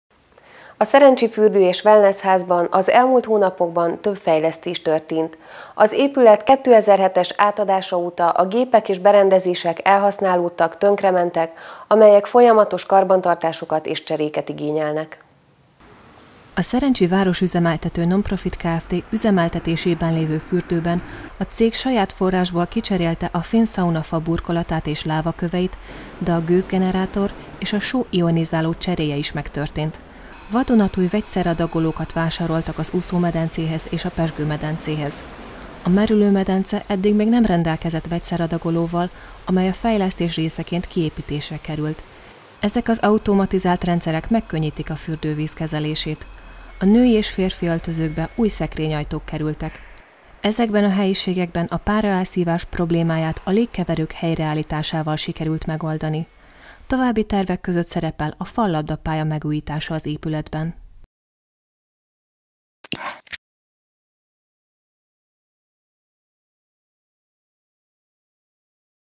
Felújítások a szerencsi fürdőben
A Szerencsi Fürdő és Wellnessházban a közelmúltban több fejlesztés történt. Az épületben 2007-ben történt teljes körű felújítás, viszont az eltelt több mint 10 év alatt az egyes berendezések elromlottak, elhasználódtak, tönkrementek.